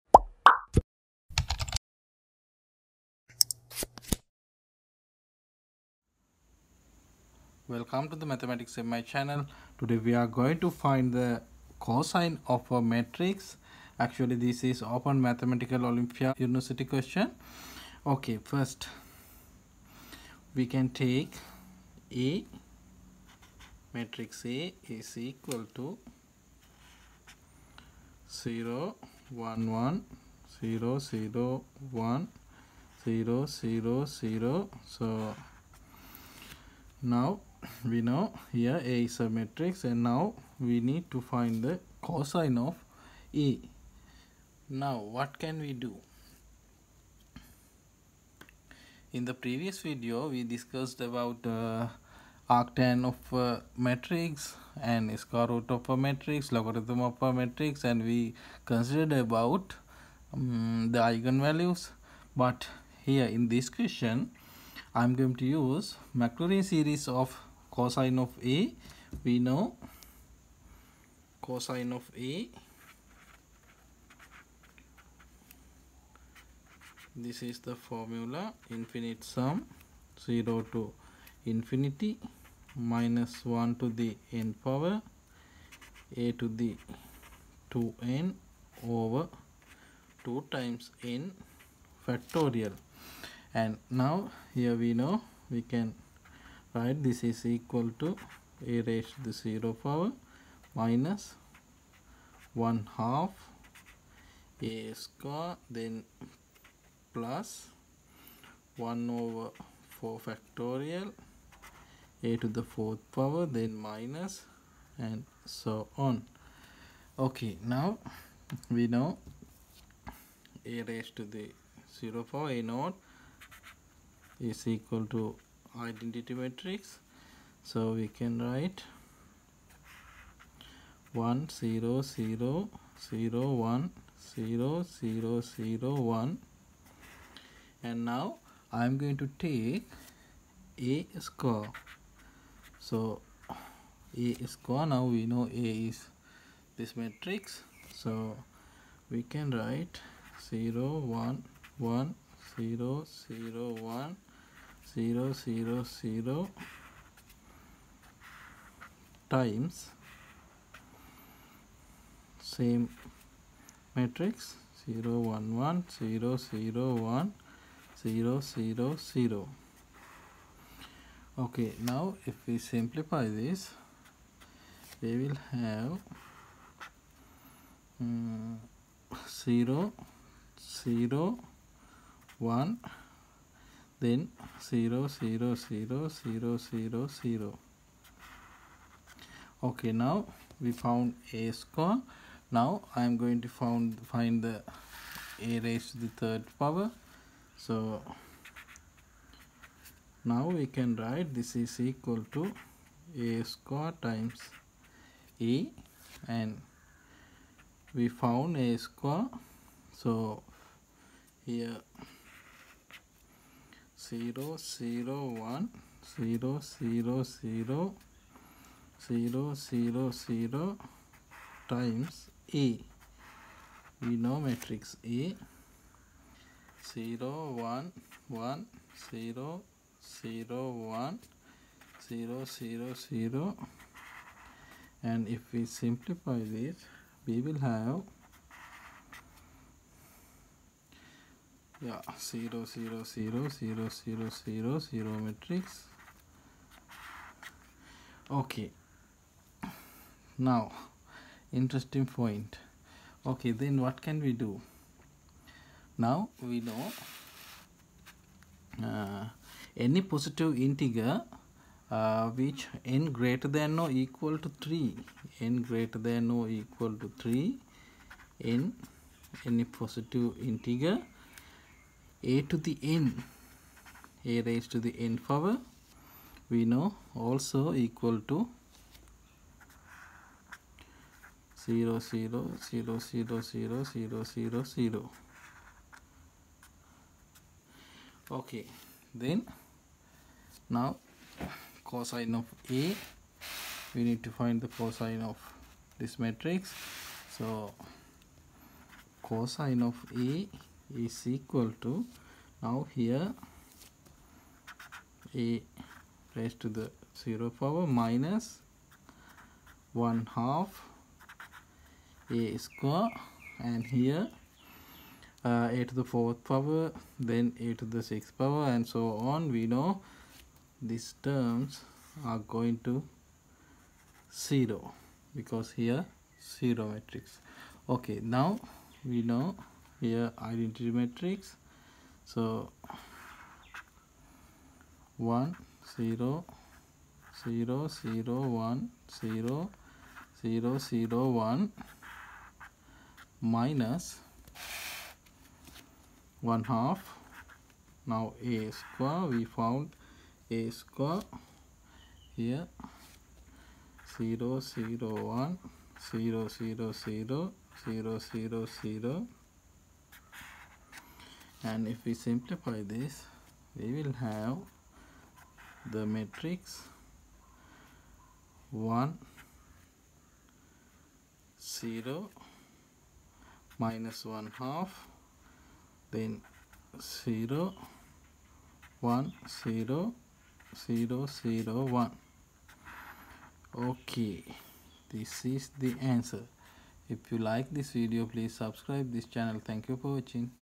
Cosine of a Matrix | Cos of a Matrix | Open Mathematical Olympiad Question Russia - 2015
This video explains about cosine of a Matrix. This is the question of Open Mathematical Olympiad Russia - 2015. I explain how to find the cosine of a Matrix.
Cosine of a Matrix | Cos of a Matrix | Linear Algebra